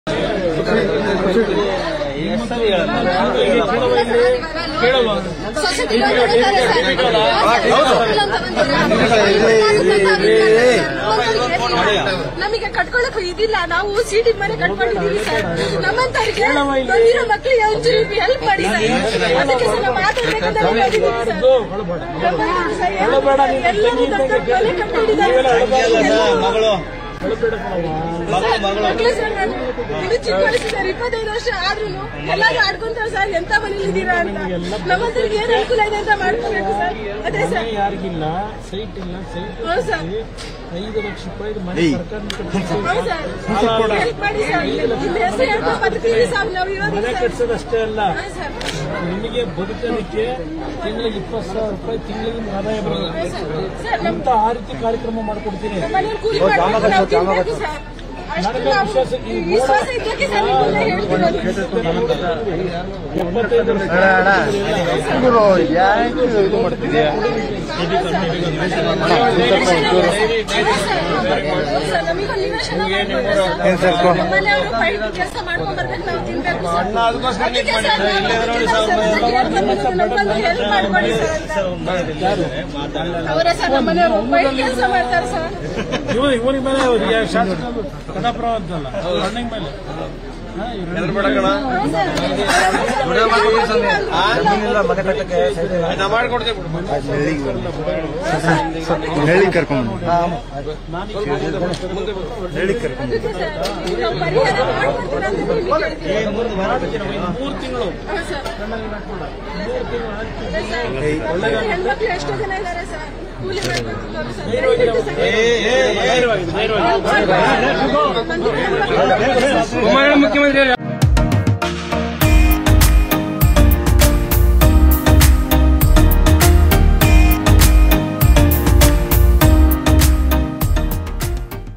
HD Kumaraswamy : ಕಣ್ಣೀರು ಹಾಕ್ಬೇಡಮ್ಮ ನಾನು ಮನೆ ಕಟ್ಟಿಸಿಕೊಡ್ತೀನಿ | Pancharatna Yatre | Power TV News
More Updates Keep Following Us